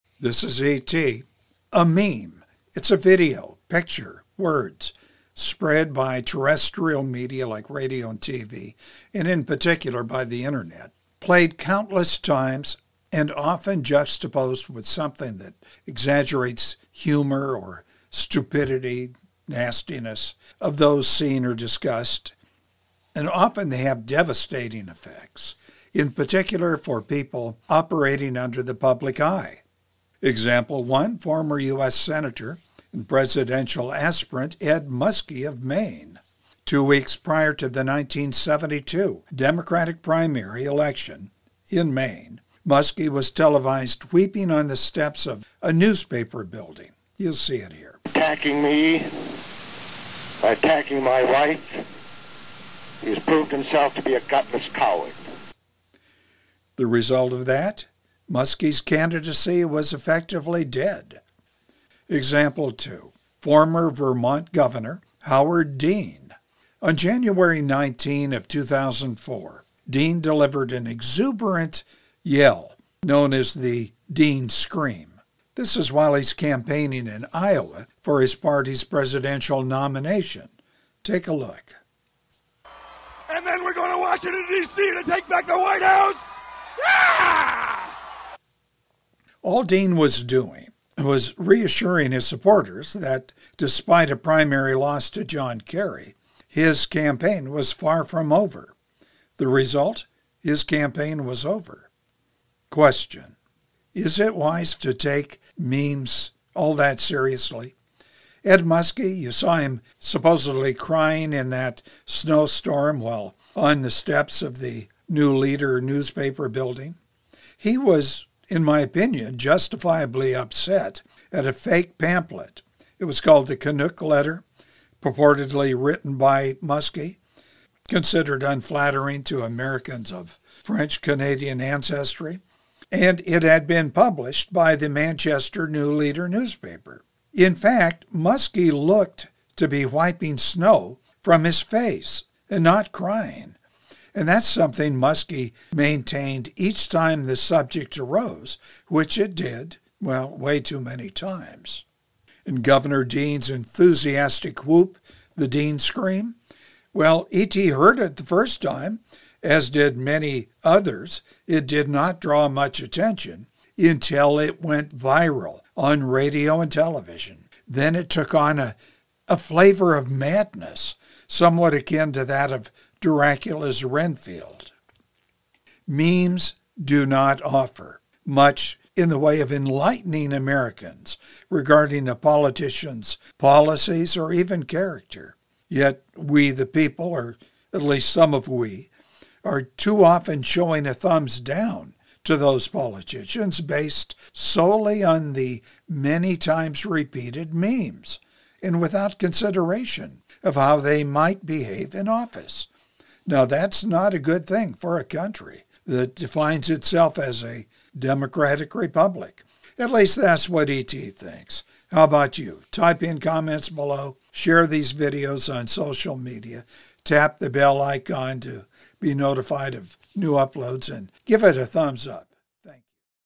DISRUPTIVE POLITICAL MEMES PT. 1
In U.S. politics, a misguided statement or out of context comment seen & heard on video can derail an aspiring politician.
This video has examples.